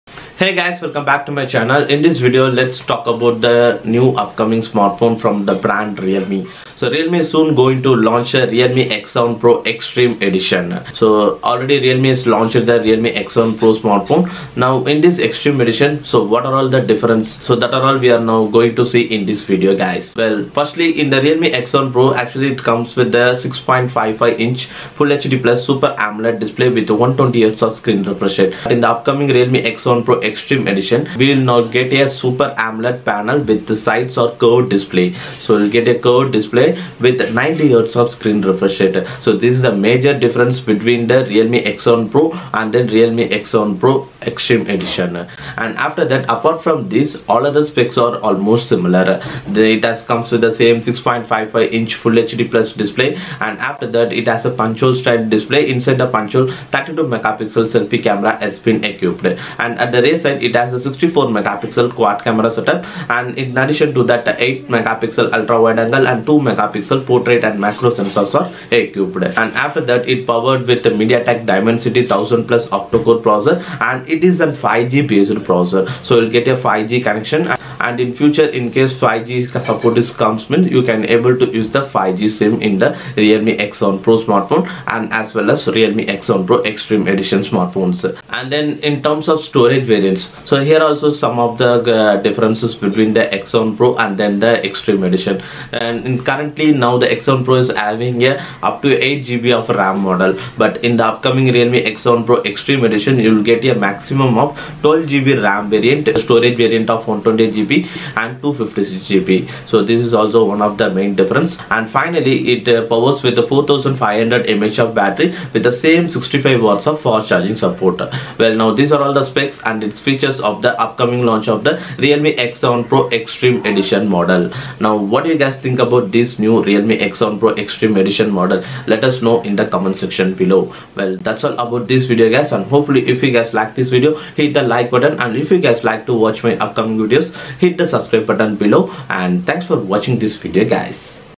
Realme X7 Pro Extreme Edition Specs 🔥 First Time Curved AMOLED Display
Realme is soon going to launch the Realme X7 Pro Extreme Edition smartphone its specifications include a Samsung Curved AMOLED display with a 90Hz screen refresh rate, a 6.55-inches display, 32MP Selfie, a 64MP Quad camera, 65W fast charging & more...